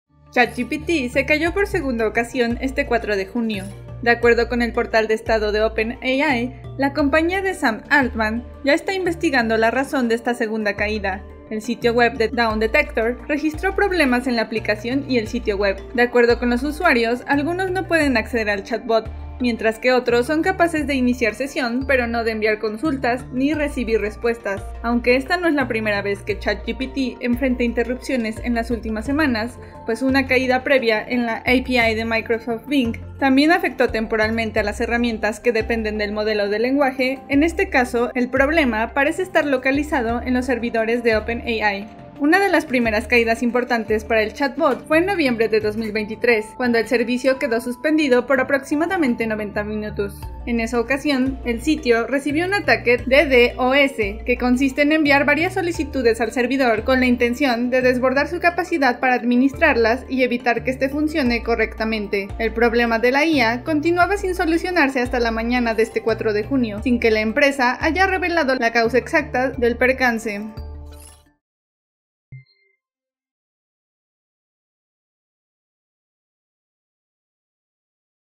ChatGPT se cae por segunda vez en un día
ChatGPT, el servicio de inteligencia artificial de OpenAI experimentó problemas por segunda vez en un solo día